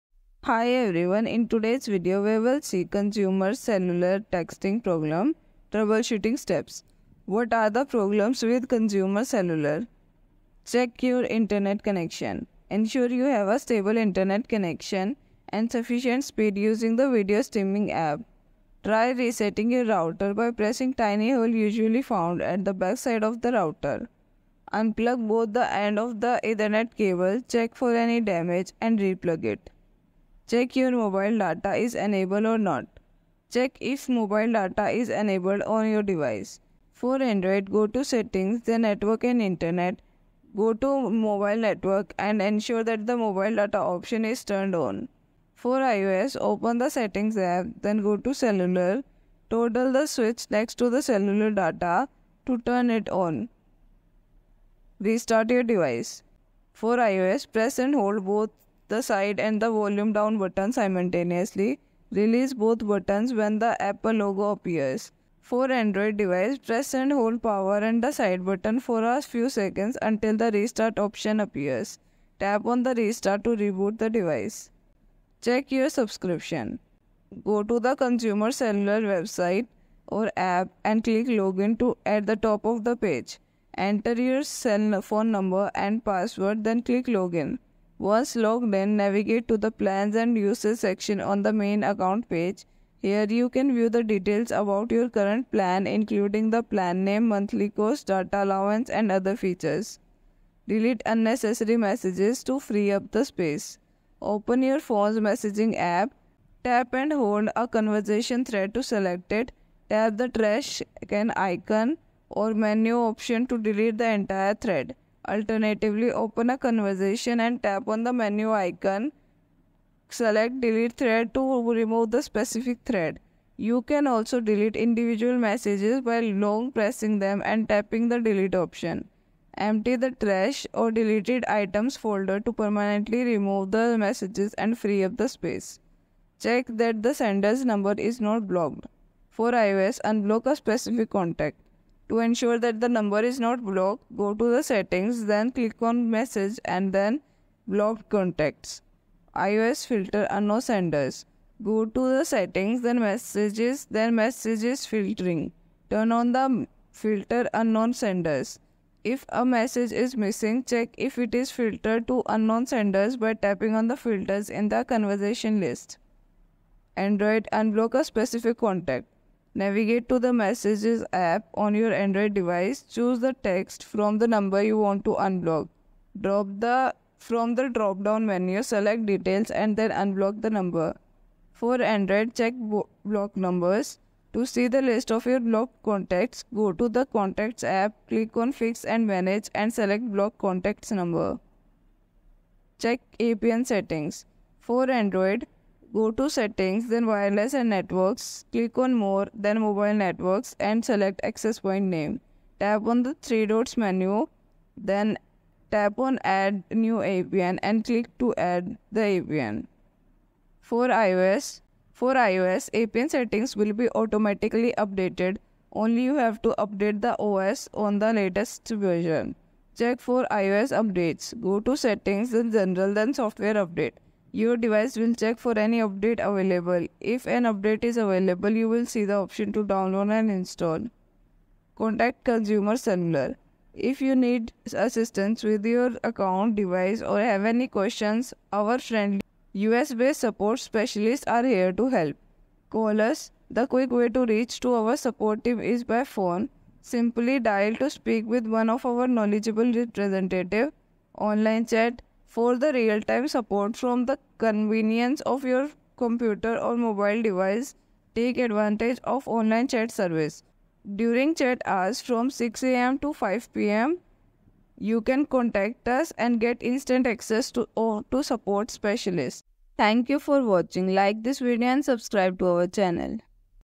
Consumer Cellular Texting Problems & Troubleshooting Steps (What Are The Problems With Consumer Cell
In this video, we will discuss common texting problems encountered by Consumer Cellular users and provide troubleshooting steps to resolve them. If you're facing issues with texting on your Consumer Cellular device, this video is a must-watch!

00:00 Intro
00:12 Step 1. Check Your Internet Connection
00:34 Step 2. Check Mobile Data Is Enabled Or Not.
00:55 Step 3. Restart Your Device
01:16 Step 4. Check Your Subscription
01:41 Step 5. Delete Unnecessary Messages To Free Up Space
02:16 Step 6. Check that The Sender's Number is Not Blocked
03:12 Step 7. Check APN Settings
03:52 Step 8. Contact Consumer Cellular